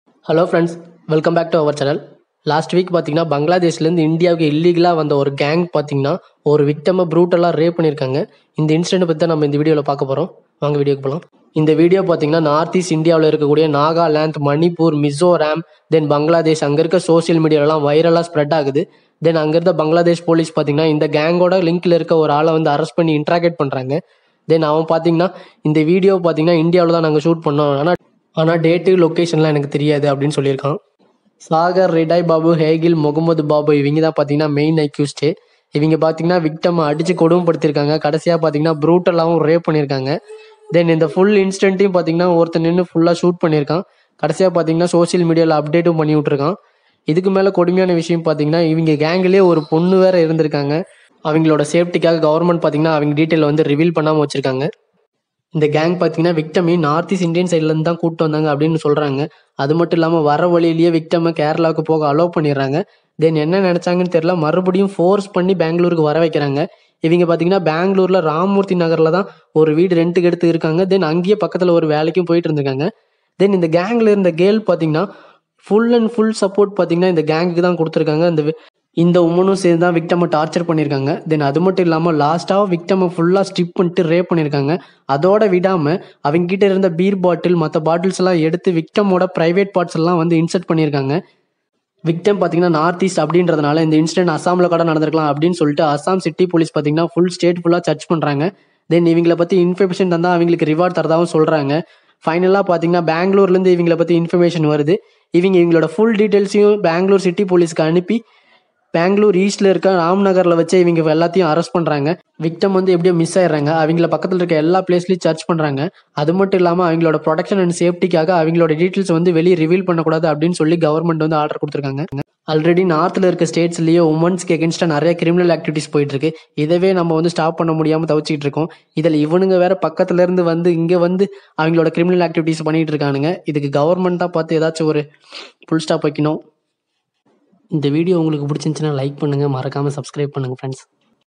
Northeast Girl | Brutal rape | Illegal bangladesh immigrants | Got arrested in East Bangalore.
Hello frds,
      The illegal bangladesh immigrants brutally raped an northeast victim in east bangalore and finally got arrested.